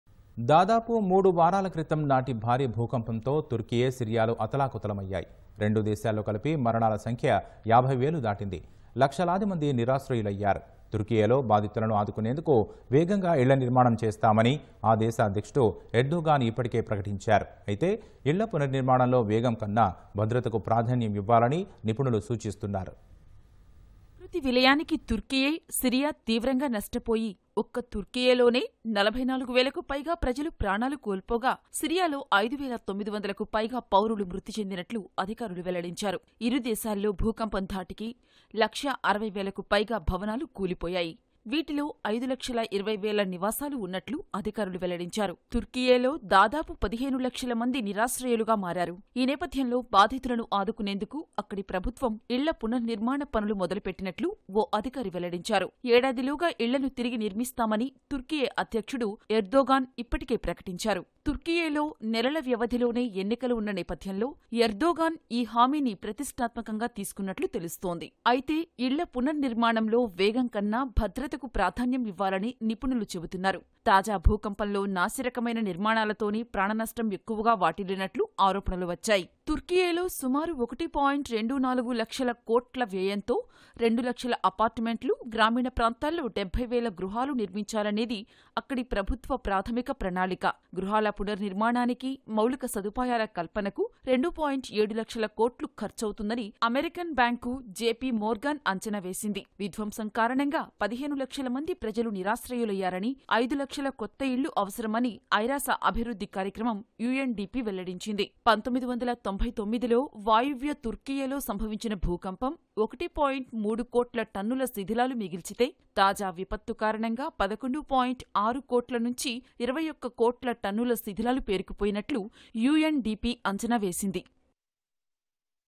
Turkey To Build Houses In Earthquake Hit Region | Prez Erdogan | తుర్కియేలో వేగంగా ఇళ్ల నిర్మాణం
దాదాపు 3 వారాల క్రితం నాటి భారీ భూకంపంతో...తుర్కియే, సిరియాలు అతలాకుతలమయ్యాయి. రెండు దేశాల్లో కలిపి మరణాల సంఖ్య...50 వేలు దాటింది. లక్షలాది మంది నిరాశ్రయులయ్యారు. తుర్కియేలో బాధితులను ఆదుకునేందుకు వేగంగా ఇళ్ల నిర్మాణం చేస్తామని...ఆదేశ అధ్యక్షుడు ఎర్దోగాన్  ఇప్పటికే ప్రకటించారు. అయితే ఇళ్ల పునర్నిర్మాణంలో వేగం కన్నా, భద్రతకు ప్రాధాన్యం ఇవ్వాలని... నిపుణులు సూచిస్తున్నారు